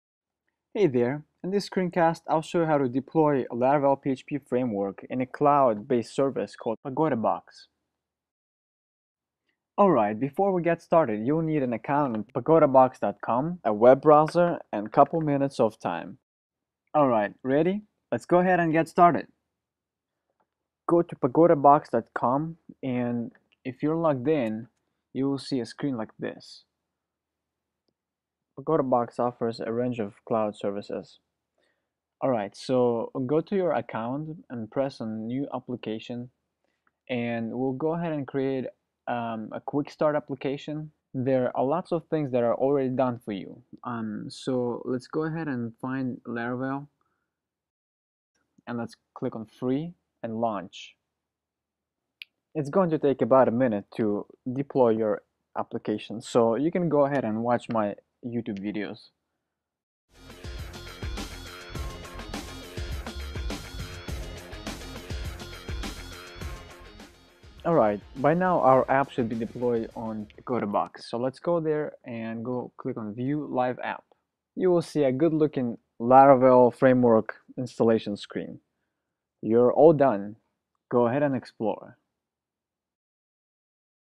Deploying Laravel on Pagodabox cloud service
Tutorial explaining how to deploy Laravel PHP framework on a cloud based service Pagodabox.

NOTE: You will need an account on PagodaBox prior to following the tutorial.